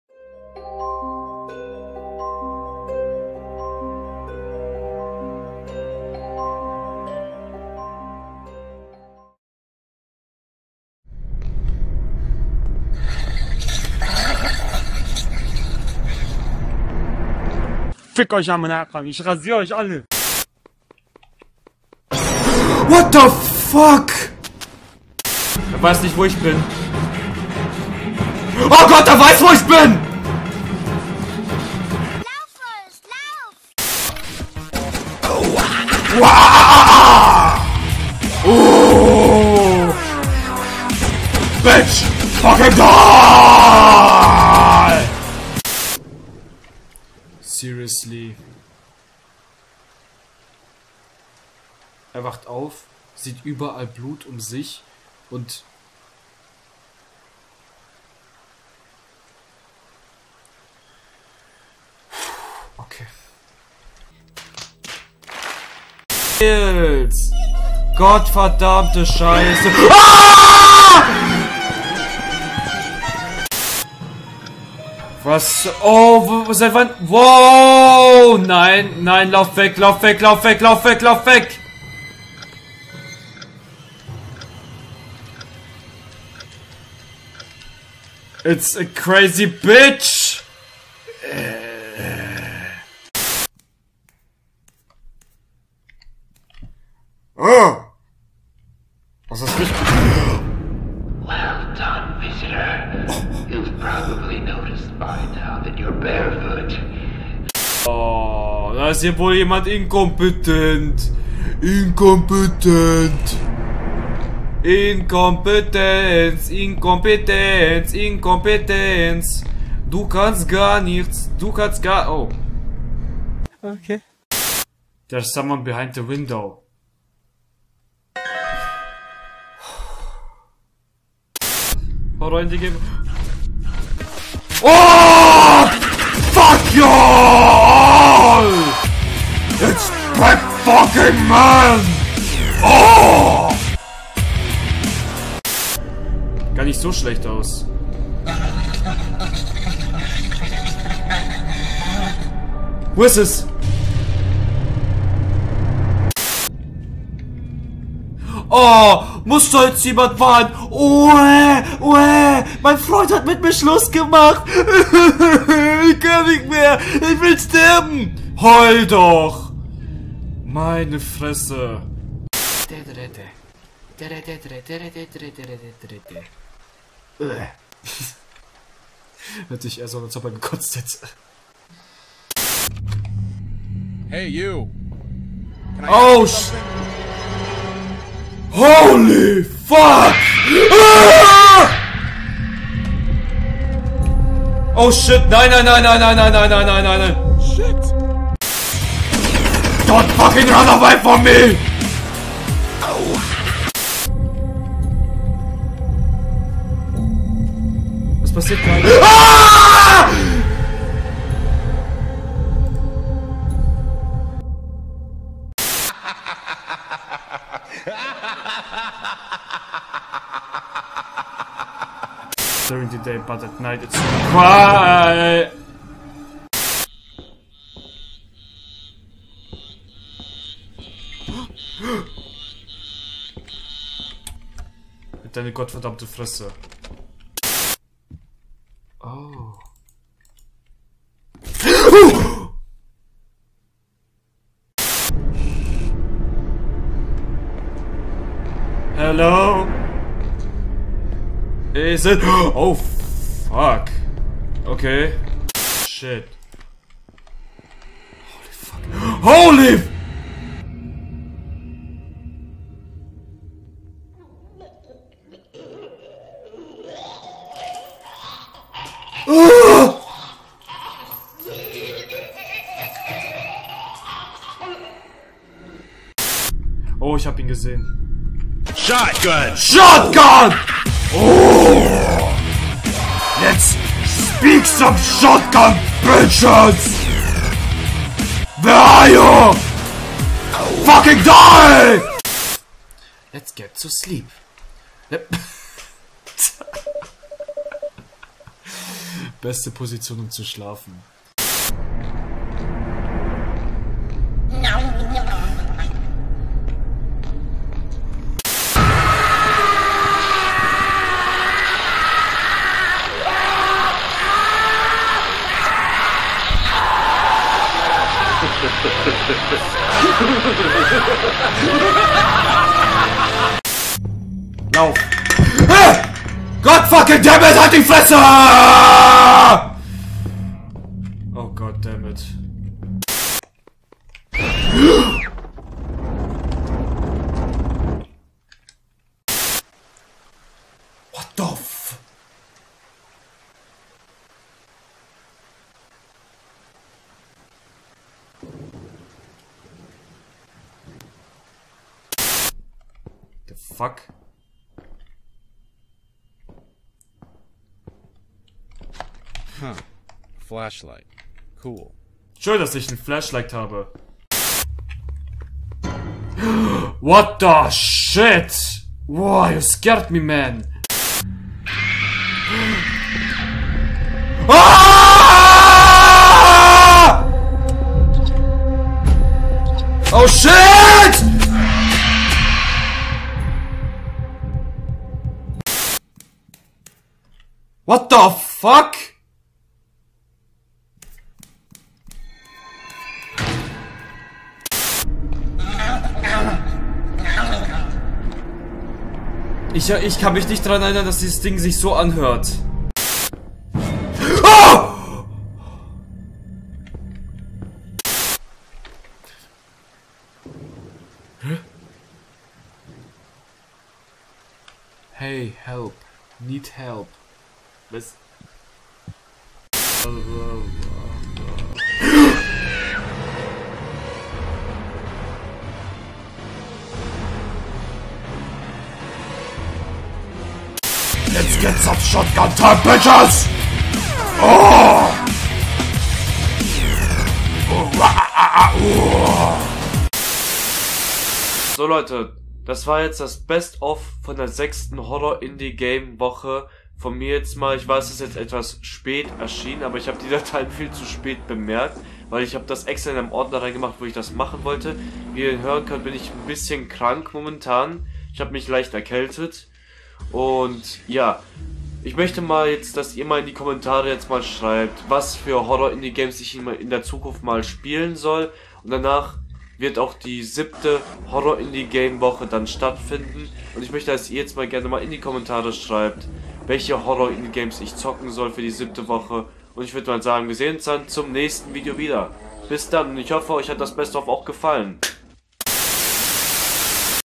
HORROR INDIE GAME WOCHE #6! | Best Of! | WTF MOMENTS!
►Hallo und herzlich willkommen zu dem sechsten Best Of von der Horror Indie Game Woche! Die ursprünglichen Videos findet ihr unten!